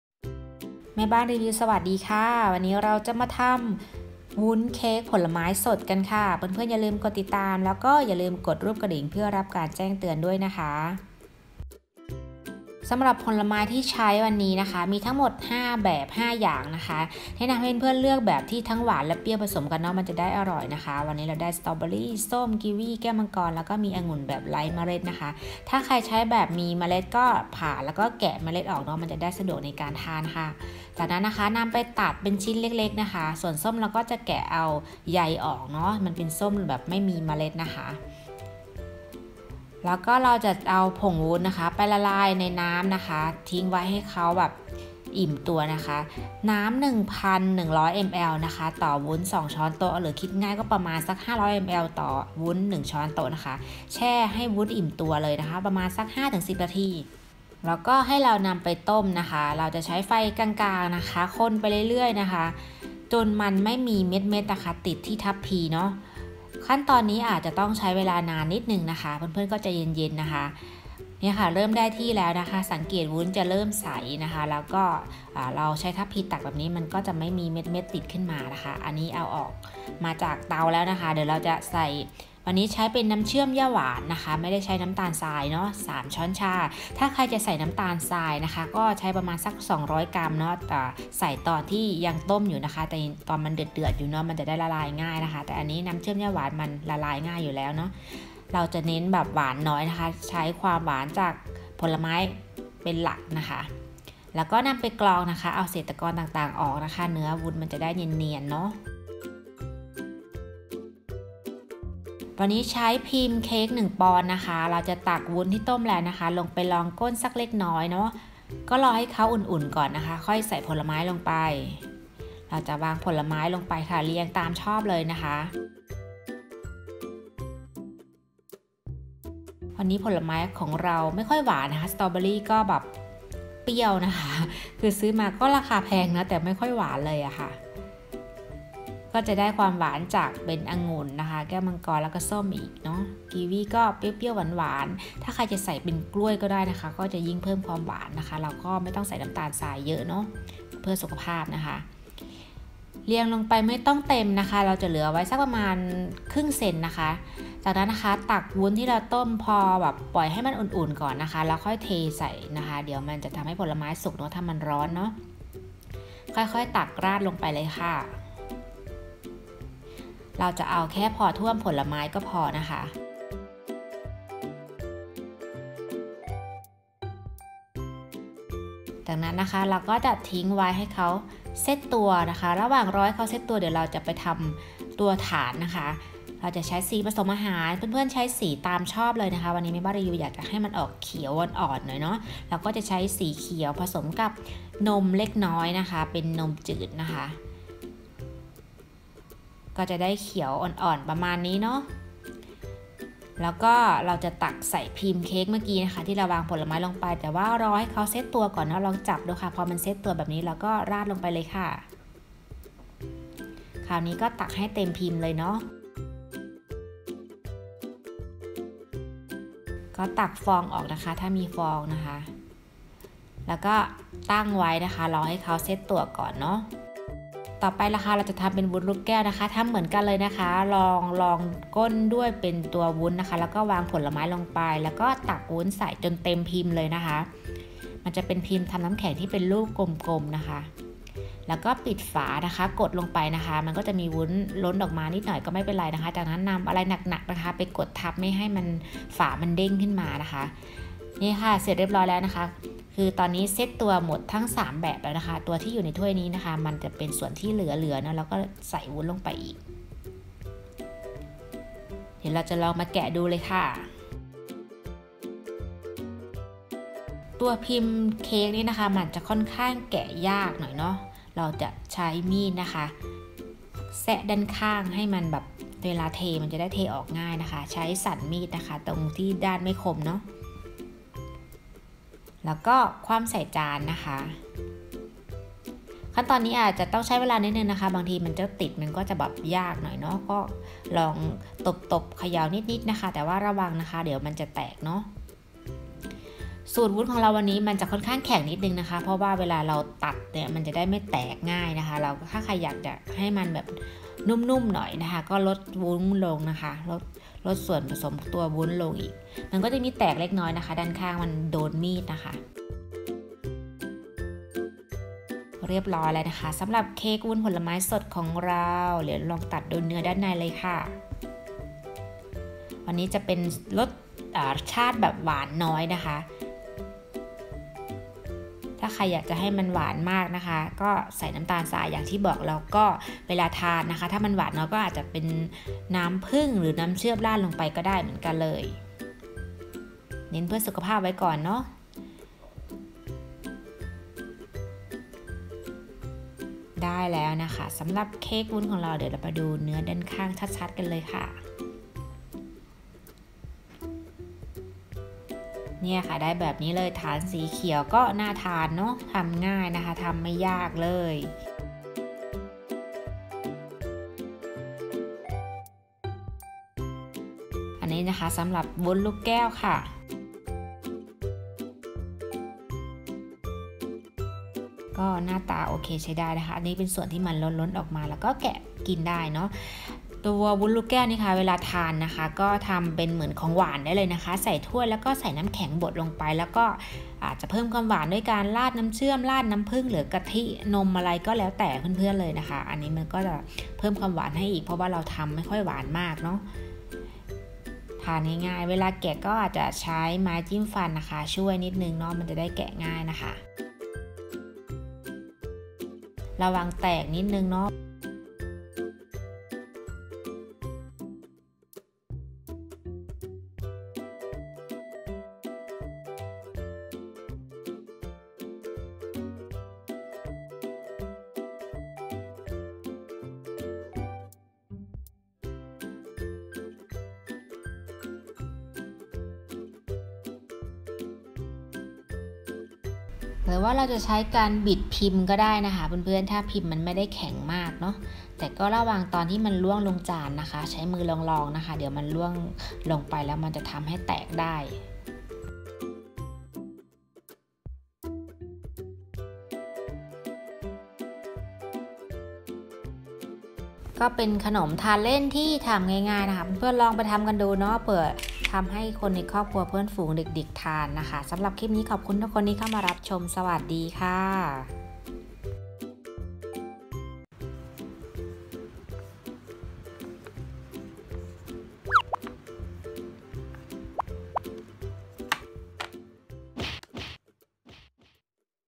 วุ้นเค้กผลไม้ วุ้นลูกแก้ว คลีน Agar Jelly Fruit Cake Jelly Bubbles | แม่บ้านรีวิว
กินผลไม้แบบธรรมดามันเบื่อ เปลี่ยนมากินแบบกรุบกรอบ อร่อยไปอีกแบบกันค่ะ หวานน้อย ดีต่อสุขภาพ ใช้หญ้าหวานแทนน้ำตาล

คลิปนี้เราทำวุ้น 2 รูปแบบ แบบเค้ก และแบบลูกแก้ว ทำไม่ยาก ไปดูส่วนผสมกันค่ะ

น้ำเปล่า 1100 ml
ผงวุ้น 2 ชต
น้ำเชื่อมหญ้าหวาน 3 ชช
ผลไม้สดตามชอบ แนะนำเลือกรสหวานผสมรสเปรี้ยว
สีผสมอาหาร 1/2 ชช
นมจืด 1 ชต

พิมพ์เค้กขนาด 1 ปอนด์
พิมพ์น้ำแข็งแบบกลม

Jelly Fruit Cake/Jelly Bubbles

Ingredients
1100 ml  Water
2 Tbsp  Agar powder
3 Tsp Stevia Syrup
Fruits - Kiwi, Strawberry, Dragon Fruit, Orange, Grape
1/2 Tsp Food Coloring Agents
1 Tbsp Fresh milk